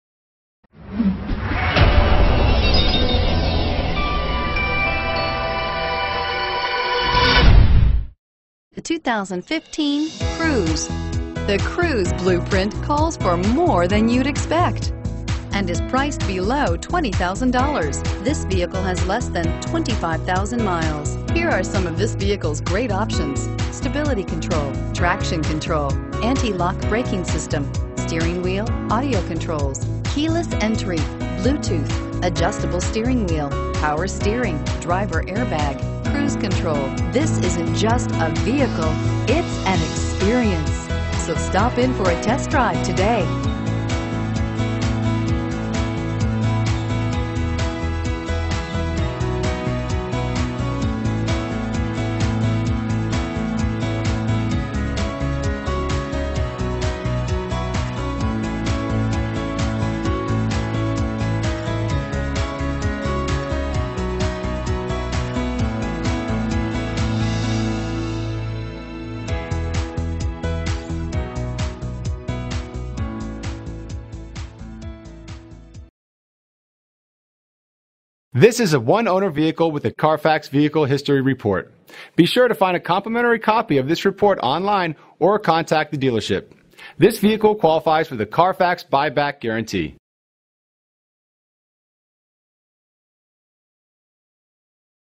2015 Chevrolet Cruze Old Bridge Township NJ C7069B
2015 Chevrolet Cruze LT

Oasis Chevrolet
1292 U.S. 9

You'll love this 2015 Chevrolet Cruze. This is a 4dr car you'll want to take home. With 24049 miles, it features Automatic transmission and an exterior color of Rainforest Green Metallic. Call and get in touch with Oasis Chevrolet directly, and be the first to open the 4dr car door today!